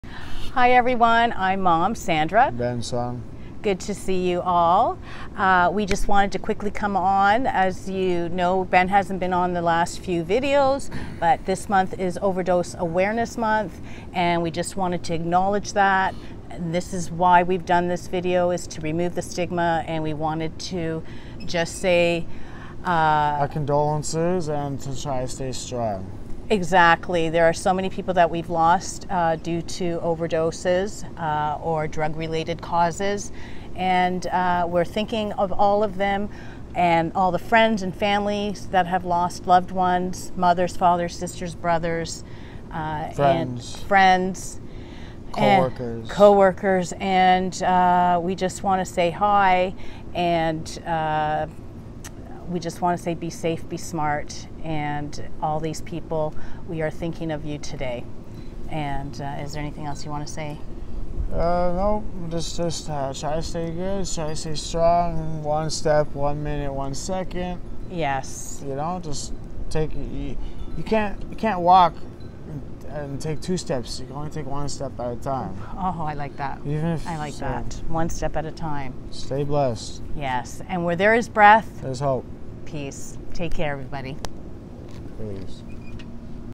Overdose Awareness
Today Ben checks in. We talk about overdose awareness and how we started this channel to share our journey and work towards removing the stigma.

Where there is breath, there is hope. ✌️ ❤️